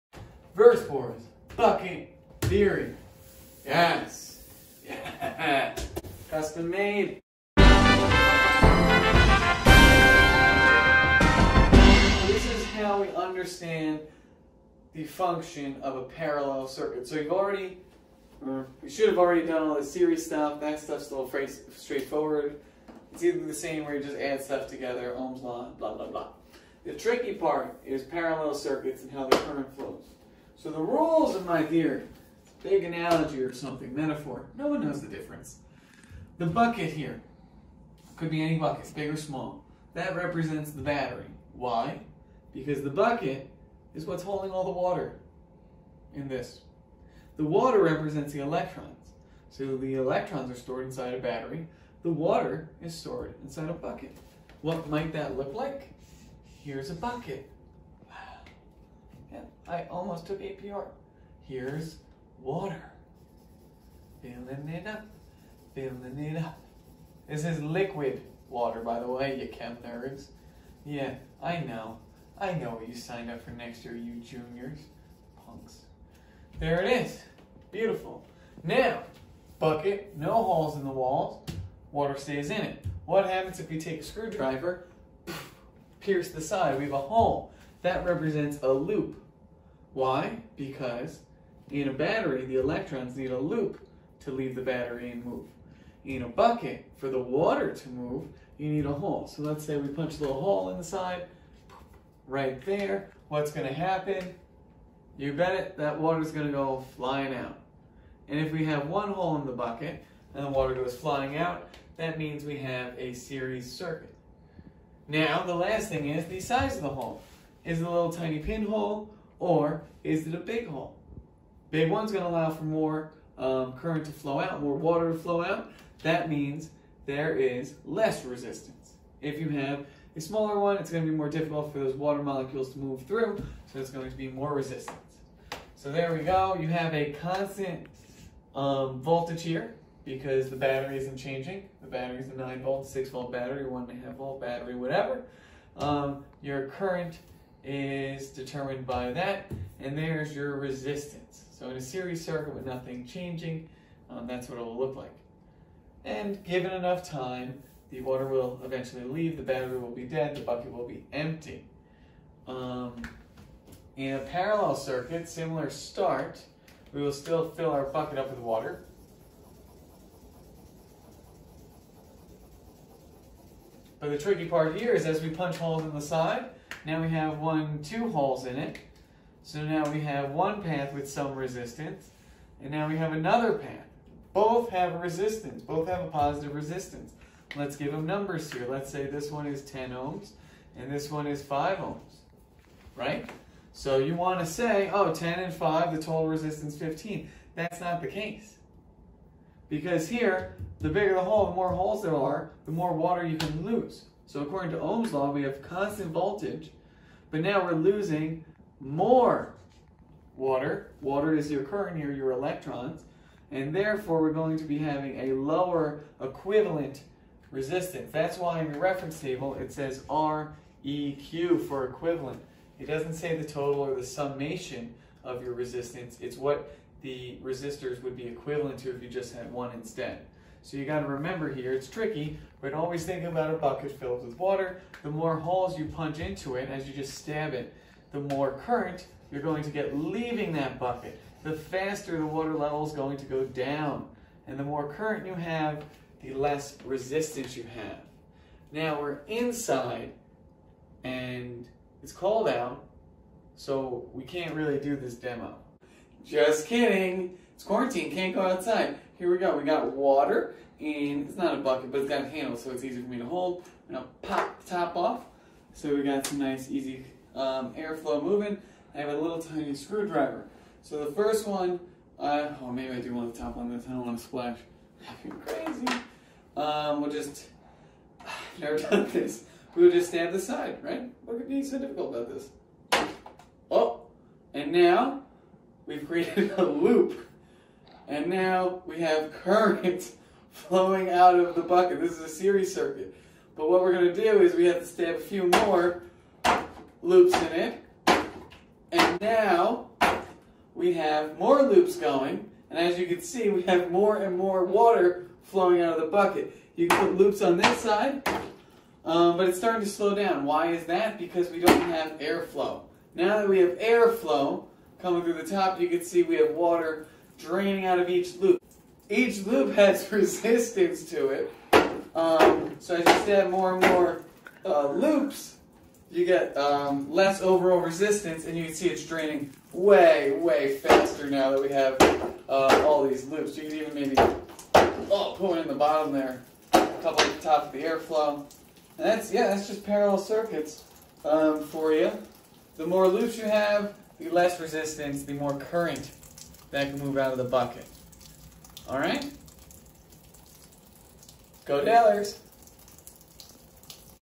Bucket Theory!!! How does adding more resistors lower the resistance of a circuit???
A one of a kind explanation of how parallel circuits create a lower equivalent resistance as more and more resistors are added.